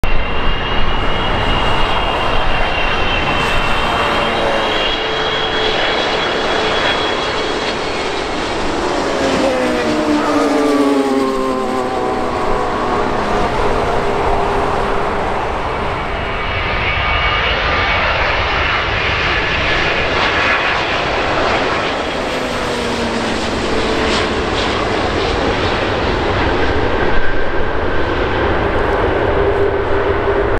AIRBUS or BOEING? Choice of plane to fly on?
(2) Virgin Atlantic flights land on runway 24R at Los Angeles International Airport (LAX) on June 2nd, 2019. Which plane would you rather fly on... the Airbus A330 or Boeing 787?

✈️ Become a AIRLINE VIDEOS First Class Member and get access to exclusive videos and perks: